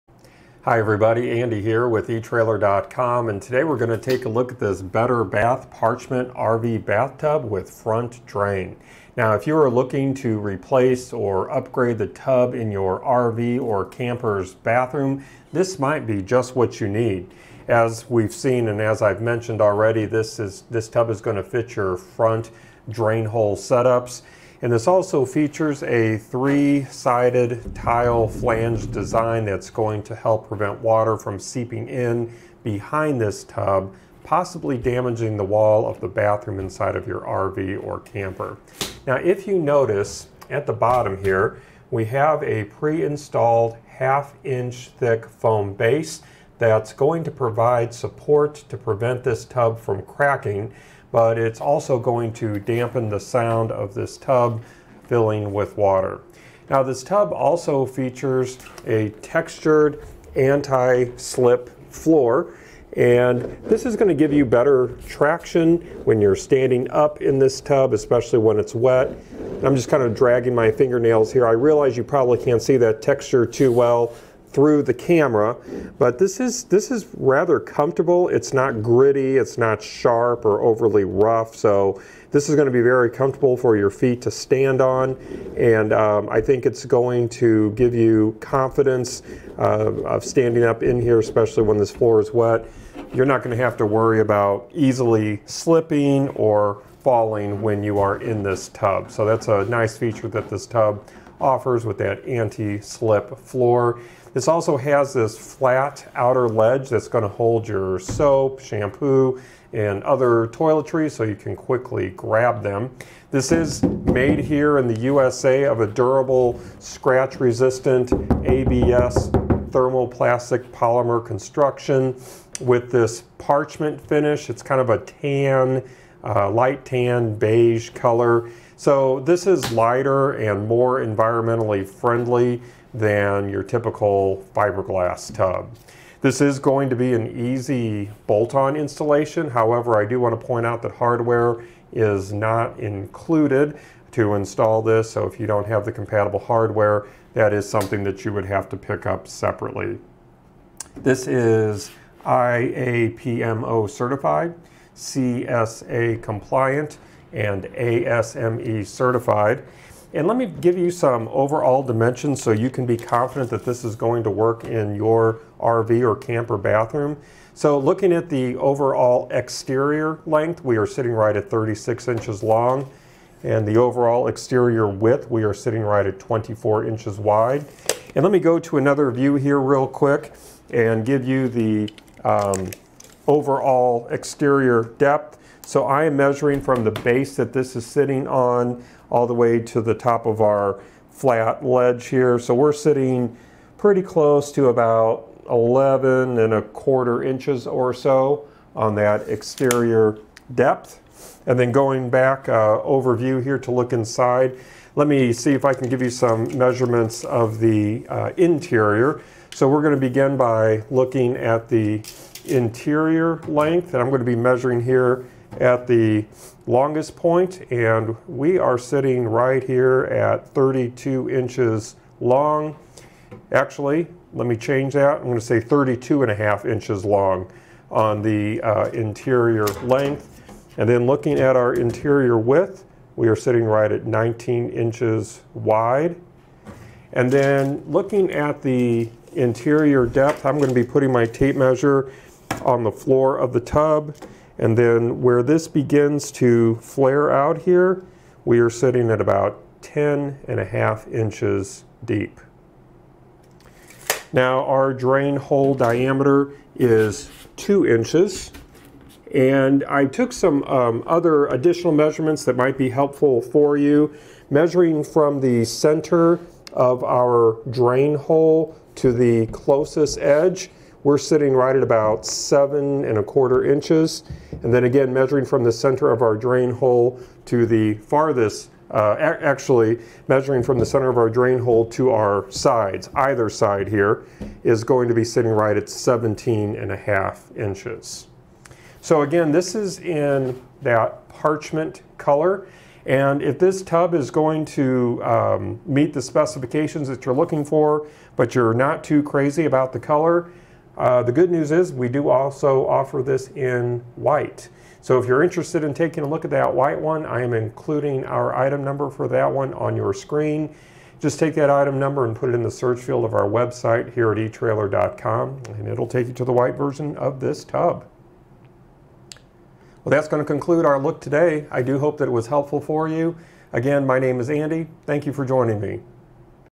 etrailer | A Closer Look at the Better Bath RV Bathtub
Click for more info and reviews of this Lippert RV Showers and Tubs:

Hi everybody, Andy here with etrailer.com, and today, we're gonna take a look at this Better Bath Parchment RV Bathtub with front drain. Now, if you are looking to replace or upgrade the tub in your RV or camper's bathroom, this might be just what you need. As we've seen and as I've mentioned already, this tub is gonna fit your front drain hole setups, and this also features a three-sided tile flange design that's going to help prevent water from seeping in behind this tub, possibly damaging the wall of the bathroom inside of your RV or camper. Now, if you noticed, at the bottom here, we have a pre-installed 1/2 inch thick foam base that's going to provide support to prevent this tub from cracking, but it's also going to dampen the sound of this tub filling with water. Now, this tub also features a textured anti-slip floor, and this is gonna give you better traction when you're standing up in this tub, especially when it's wet. I'm just kinda dragging my fingernails here, I realized you probably can't see that texture too well through the camera, but this is rather comfortable, it's not gritty, it's not sharp or overly rough, so this is gonna be very comfortable for your feet to stand on, and I think it's going to give you confidence of standing up in here, especially when this floor is wet, you're not gonna have to worry about easily slipping or falling when you are in this tub, so that's a nice feature that this tub offers with that anti-slip floor.

This also has this flat outer ledge that's gonna hold your soap, shampoo, and other toiletries, so you can quickly grab them. This is made here in the USA of a durable scratch resistant ABS thermoplastic polymer construction with this parchment finish, it's kind of a tan, light tan, beige color, so this is lighter and more environmentally friendly than your typical fiberglass tub. This is going to be an easy bolt-on installation. However, I do wanna point out that hardware is not included to install this, so if you don't have the compatible hardware, that is something that you would have to pick up separately. This is IAPMO certified, CSA compliant, and ASME certified.

And let me give you some overall dimensions, so you can be confident that this is going to work in your RV or camper bathroom. So looking at the overall exterior length, we are sitting right at 36 inches long, and the overall exterior width, we are sitting right at 24 inches wide, and let me go to another view here real quick and give you the overall exterior depth, so I am measuring from the base that this is sitting on all the way to the top of our flat ledge here, so we're sitting pretty close to about 11 1/4 inches or so on that exterior depth. And then going back overview here to look inside, let me see if I can give you some measurements of the interior. So we're gonna begin by looking at the interior length, and I'm gonna be measuring here at the longest point, and we are sitting right here at 32 inches long. Actually, let me change that, I'm gonna say 32 1/2 inches long on the interior length.

And then looking at our interior width, we're sitting right at 19 inches wide, and then looking at the interior depth, I'm gonna be putting my tape measure on the floor of the tub and then where this begins to flare out here, we're sitting at about 10 1/2 inches deep. Now, our drain hole diameter is two inches, and I took some other additional measurements that might be helpful for you. Measuring from the center of our drain hole to the closest edge, we're sitting right at about 7 1/4 inches, and then again, measuring from the center of our drain hole to the farthest, actually, measuring from the center of our drain hole to our sides, either side here, is going to be sitting right at 17 1/2 inches. So again, this is in that parchment color, and if this tub is going to meet the specifications that you're looking for, but you're not too crazy about the color, the good news is we do also offer this in white, so if you're interested in taking a look at that white one, I am including our item number for that one on your screen, just take that item number and put it in the search field of our website here at etrailer.com, and it'll take you to the white version of this tub. Well, that's gonna conclude our look today, I do hope that it was helpful for you.

Again, my name is Andy, thank you for joining me..